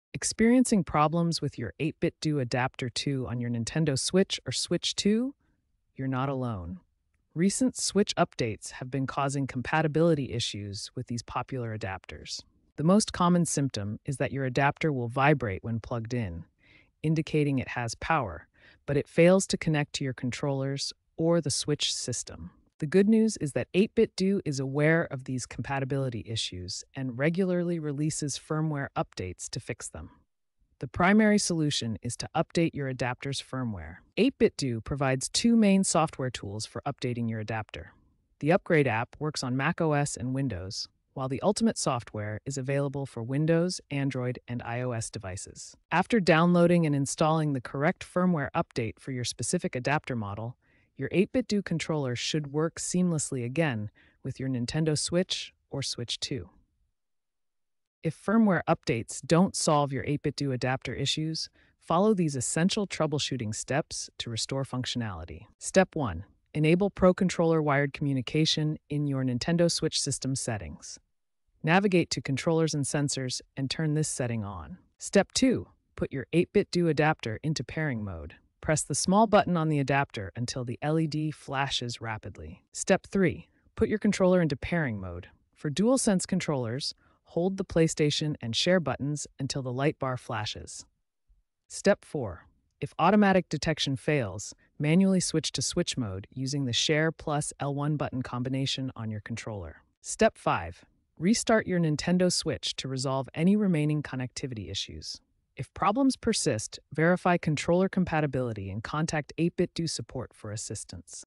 How to Fix 8BitDo Adapter 2 Not Working on Switch – Mode Switch, Firmware & Pairing Fix
In this comprehensive tutorial, we will guide you through the process of fixing the 8BitDo Adapter 2 when it fails to work with your Nintendo Switch. Whether you're experiencing issues with mode switching, firmware updates, or pairing your devices, this video covers all the essential troubleshooting steps you need to get your adapter functioning smoothly again. Follow along as we provide clear instructions and helpful tips to ensure a seamless gaming experience. Don't let connectivity issues hold you back—watch now and get back to gaming!